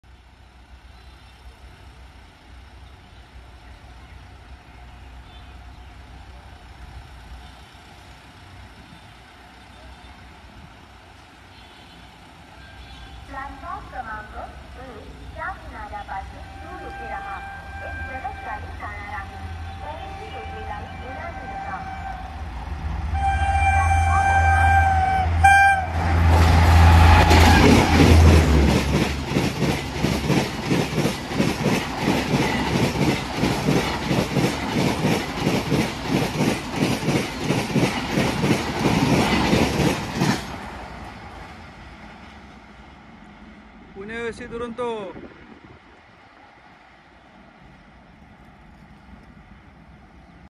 Witness the Fury: Aggressive 130KMPH Pune Duronto Express
12263 Pune - Hazrat Nizamuddin AC Duronto Express with TKD WAP7 30432
Watch out my another videos giving links